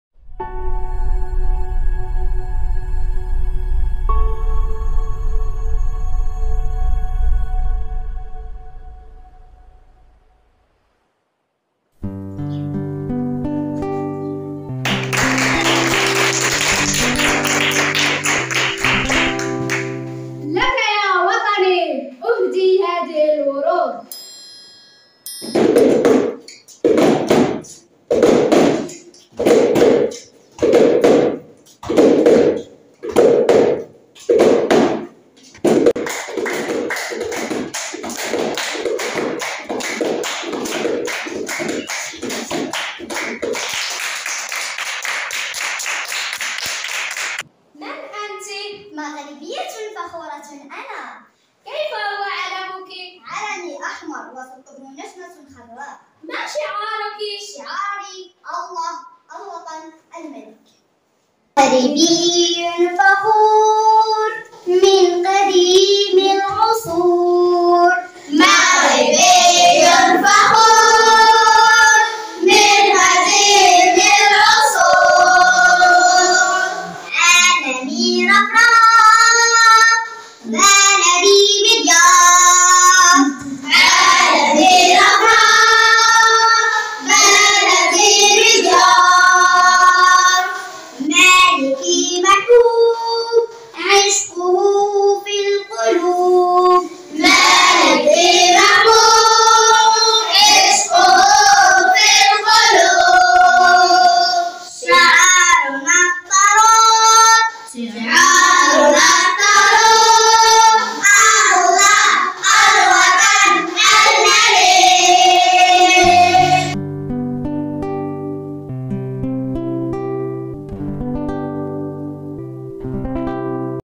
ملحمة المغاربة مسرحية للمستوى الخامس ،الجديد في التربية الفنية2022\2023(درس للإستئناس)
من تأطير وإشراف:الإدارة التربوية( ل م.م إرزي)هيئة التفتيش بمديرية تارودانت.
الهدف من النشاط تقريب وتقاسم التجارب بغيت التنقيح والتصويب.